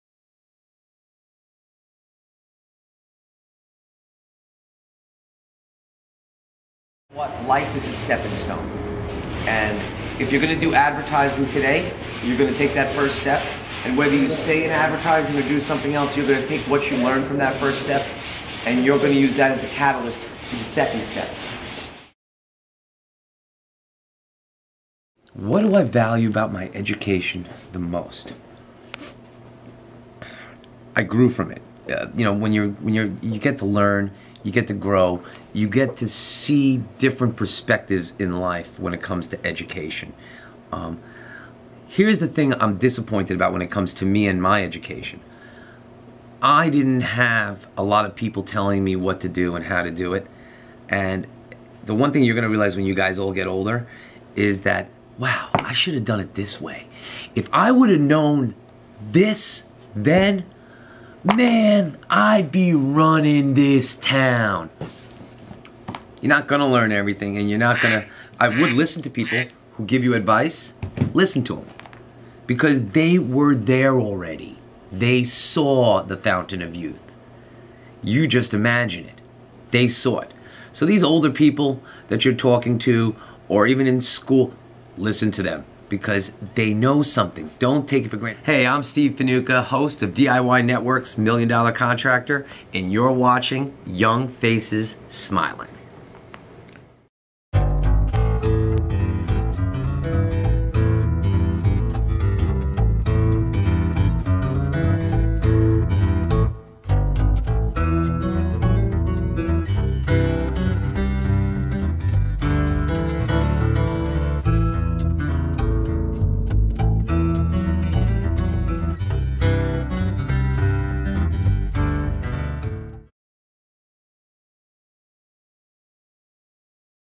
Stephen Fanuka with Young Faces Smiling
TV Show - Million Dollar Contractor

A Reality Classroom Adventure!

Interviewed By: Students - Donte Lewis and Carmen Diaz
Date:

Young Faces Smiling
455 Tarrytown Road
White Plains, New York 10607
*** 501c(3) non-profit organization